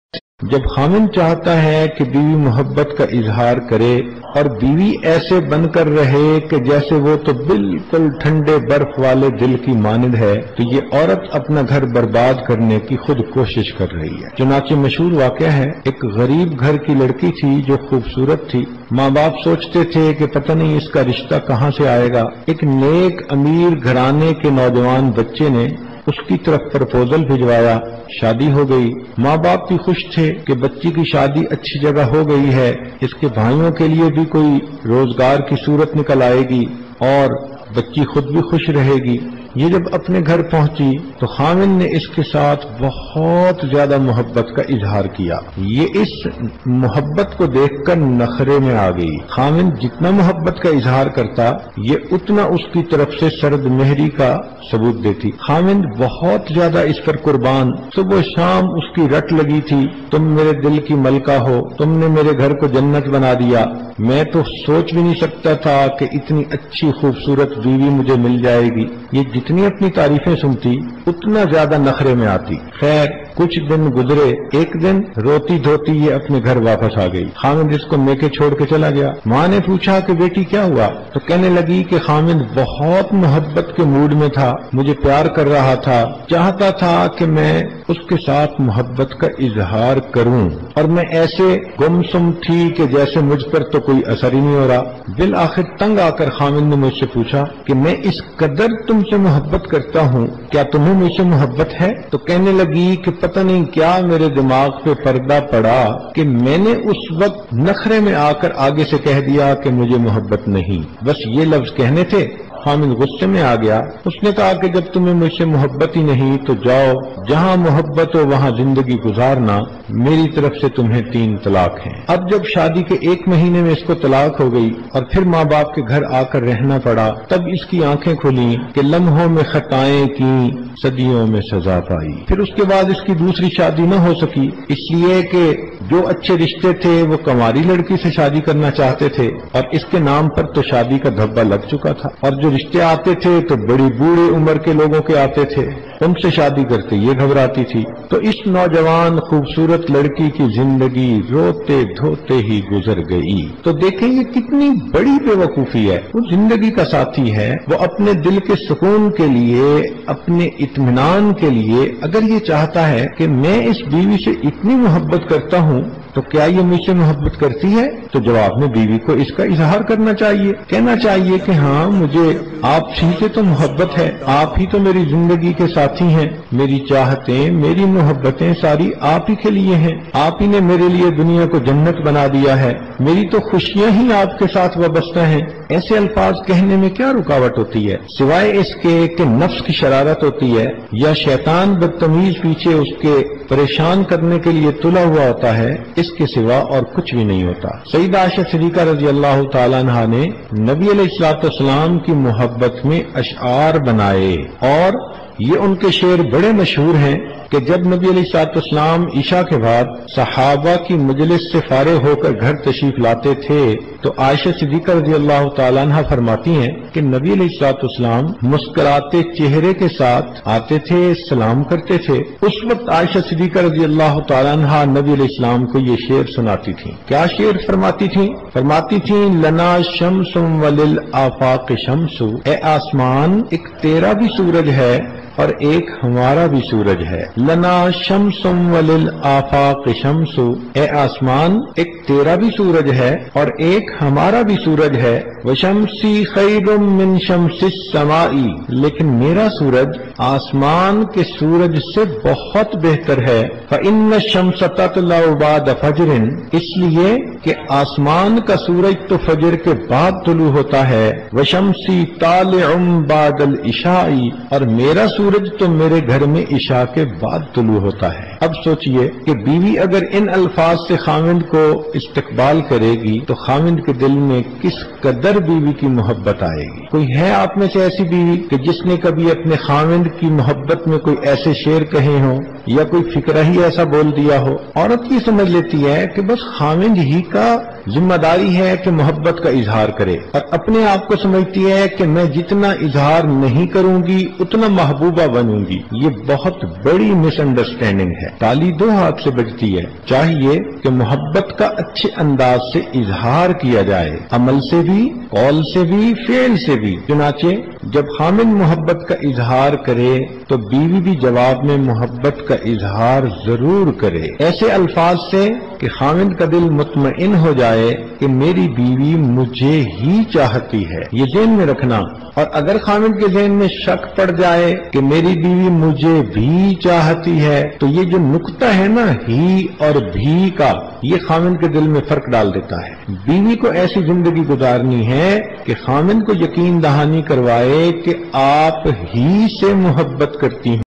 Aouratain Apnay Gharoon Ko Kaisay Barbad Karti Hain Molana Peer Zulfiqar Ahmad Naqshbandi Sahab عورت
Aouratain Apnay Gharoon Ko Kaisay Barbad Karti Hain Molana Peer Zulfiqar Ahmad Naqshbandi Sahab

 عورتیں اپنے گھروں کو کیسے برباد کرتی ہیں؟ مولانا پیر ذوالفقار احمد نقشبندی صاحب